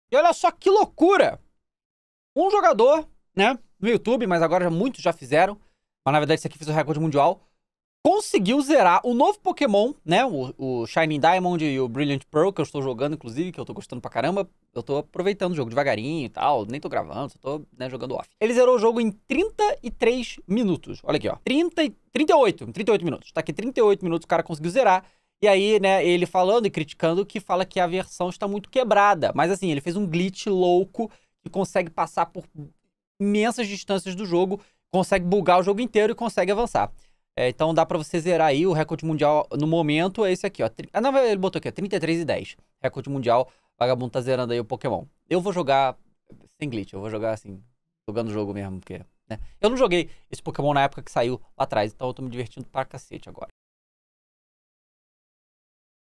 ZERANDO UM JOGO O MAIS RÁPIDO POSSÍVEL
Fala galera! No vídeo de hoje teremos um corte comentando sobre o que os Speedrunners estão fazendo com o novo jogo do Pokémon.